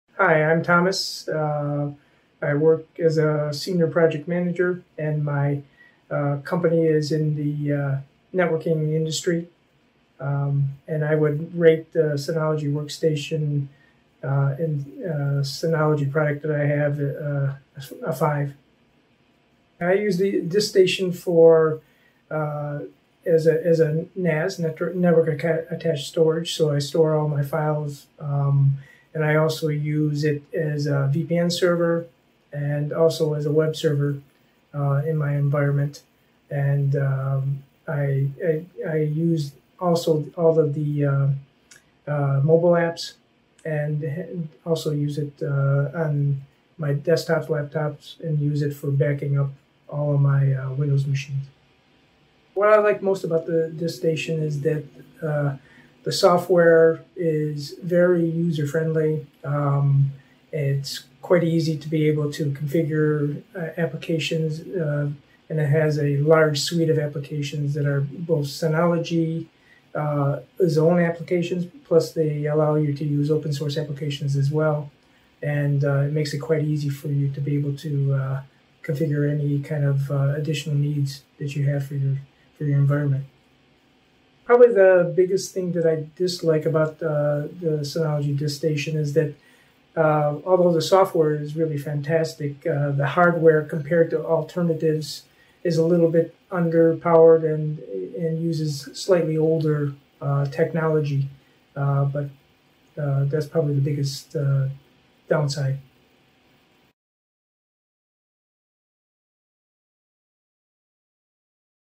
DiskStation Review: Excellent Value
"💡📘 Interested in DiskStation? Browse DiskStation reviews, pricing and compare with popular alternatives

DiskStation Review 2023.

With Virtual Machine Manager (VMM) you can set up and run various virtual machines, including Windows and Linux, on Synology NAS..

Virtual Machine Manager opens up abundant possibilities. You can set up and run various virtual machines, including Windows, Linux, and Ubuntu, on one Synology NAS. Easily allocate hardware resources such as computing power, storage space, or network interfaces among several virtual machines. Clone, restore, or create a failover of virtual machines within seconds, and rest assured that you can rollback any failure at any time.

0:00 Intro
0:19 What business problems are you solving with DiskStation?
0:52 What do you like most about DiskStation?
1:24 What do you like least about DiskStation?"